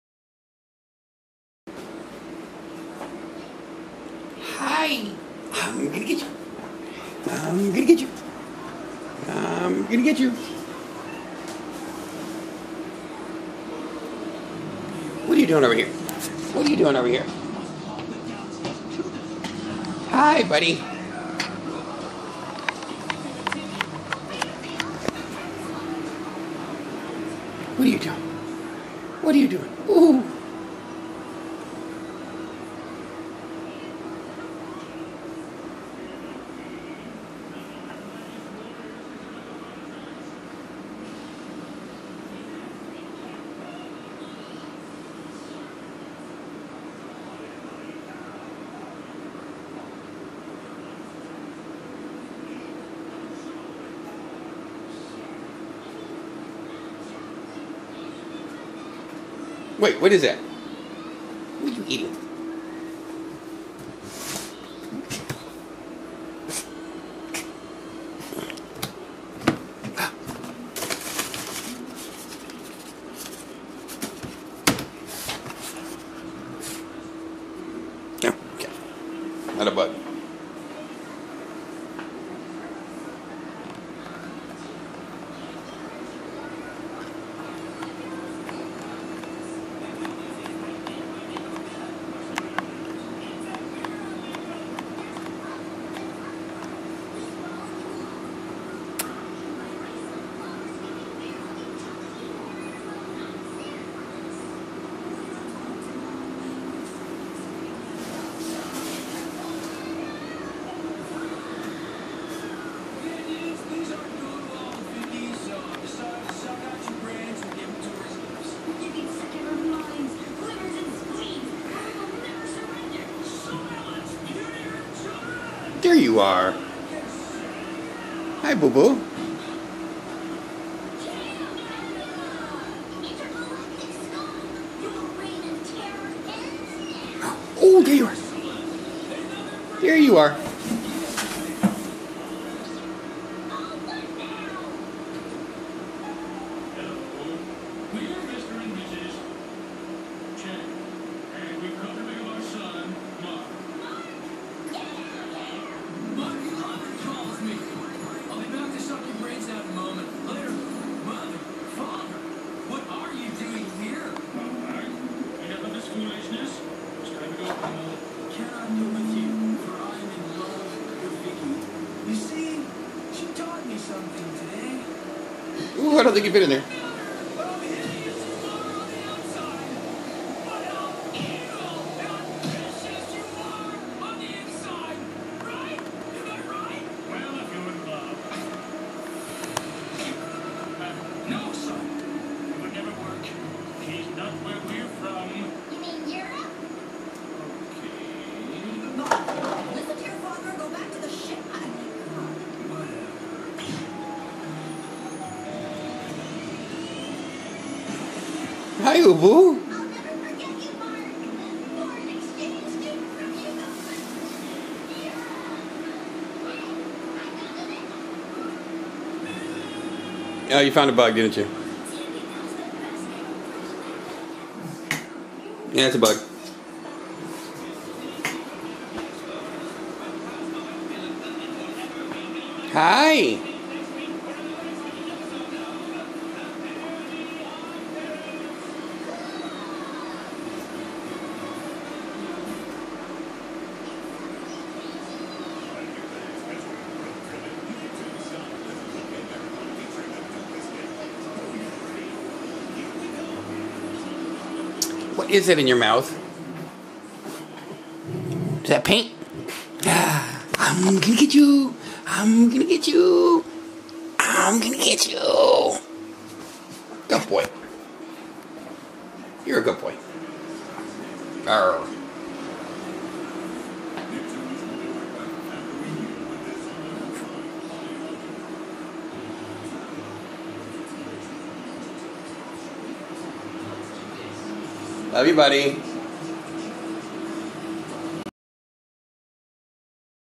Loki the red fox sniffin around the house
Just a typical day with my fox :)